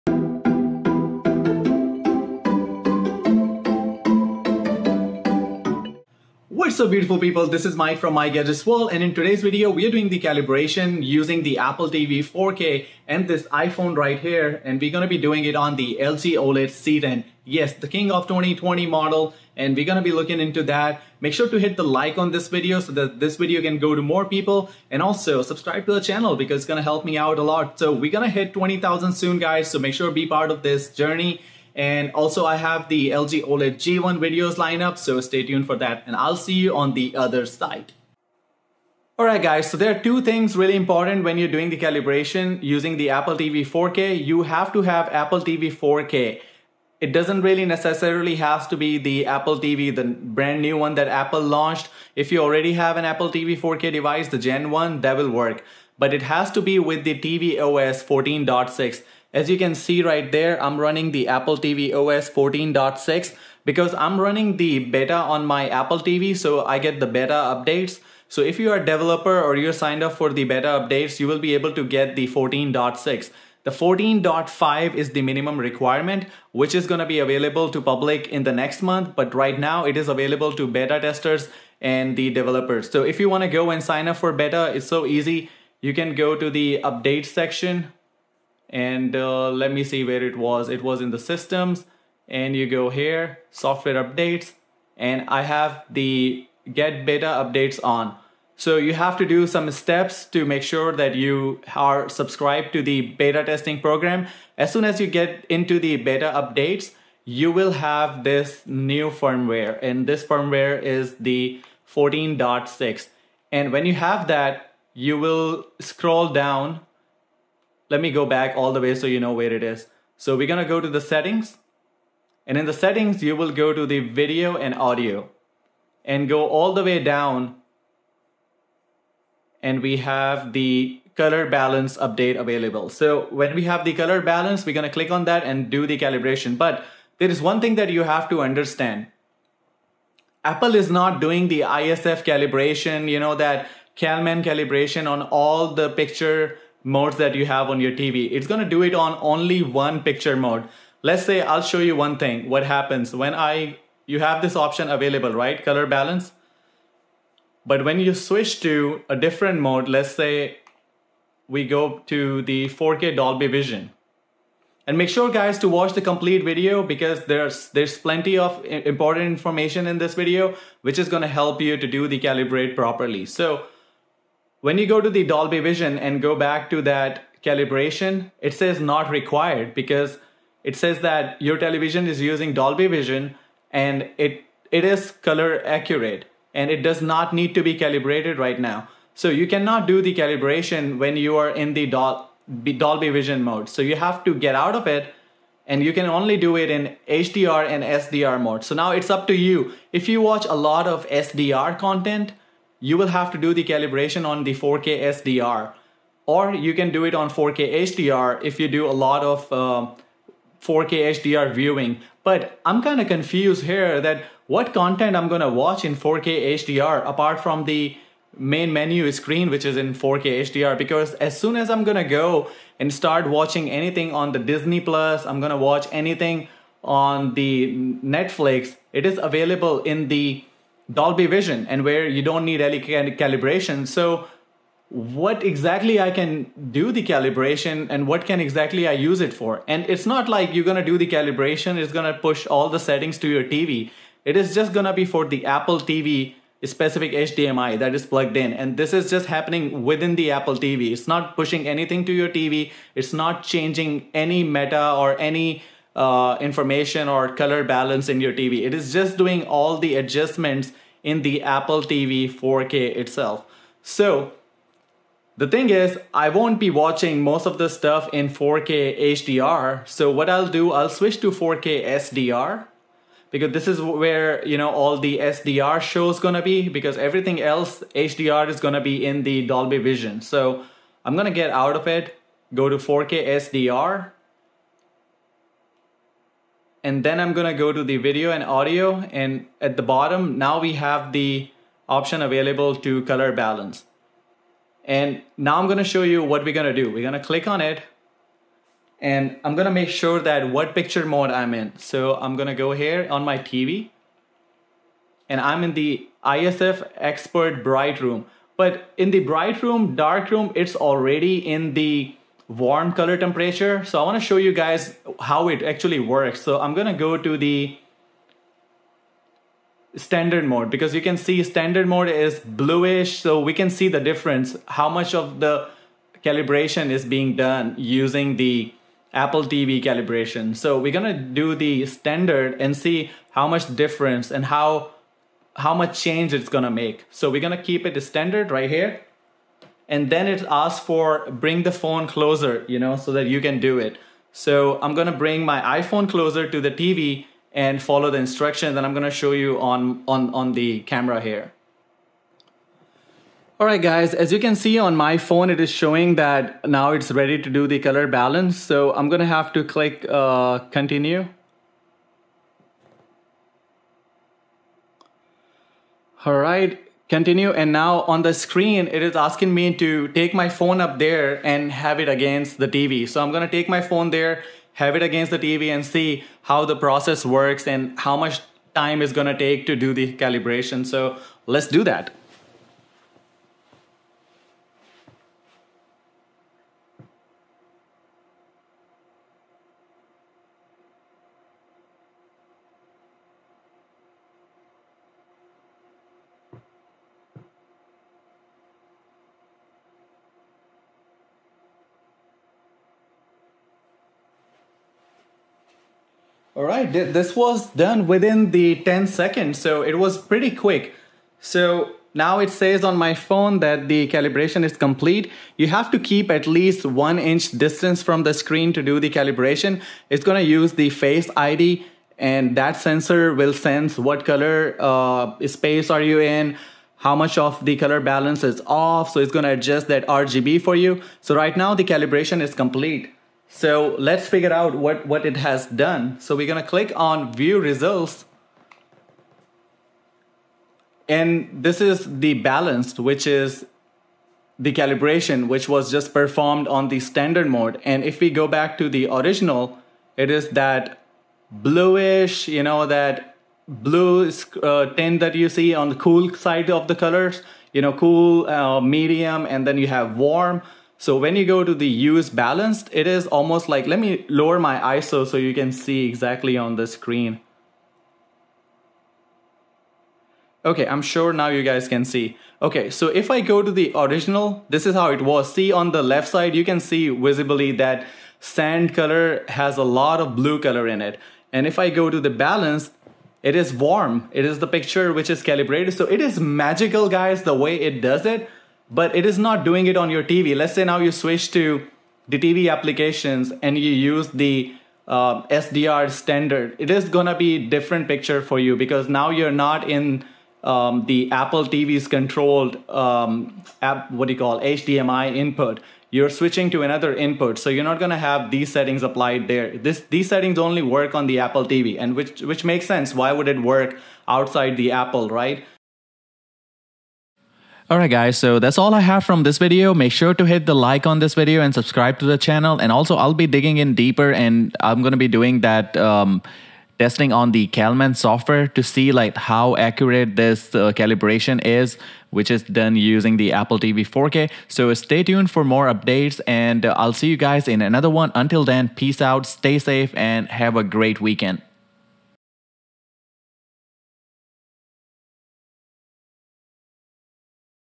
Calibrating LG OLED using Apple TV 4K new color balance calibration feature
Calibrating LG OLED using Apple TV 4K new color balance calibration feature , I used my LG OLED CX 77 inch TV for this demonstration. It does work and you can see the results before and after. This feature is available on Apple TV 4K 1st Gen, so you will not have to buy the brand new Apple TV 4K to do it.
0:00 Intro
0:10 What are we using to do calibration
00:40 Software/firmware information Apple tvOS 14.5 minimum
1:30 Sign up for apple tvOS Beta
2:00 Apple TV 4K Color balance calibration menu
2:30 Dolby Vision cannot be calibrated using color balance
3:15 4K SDR / 4K HDR Calibration
5:00 Start Calibration
5:55 iPhone 11 with iOS 14.5
7:20 Before and after calibration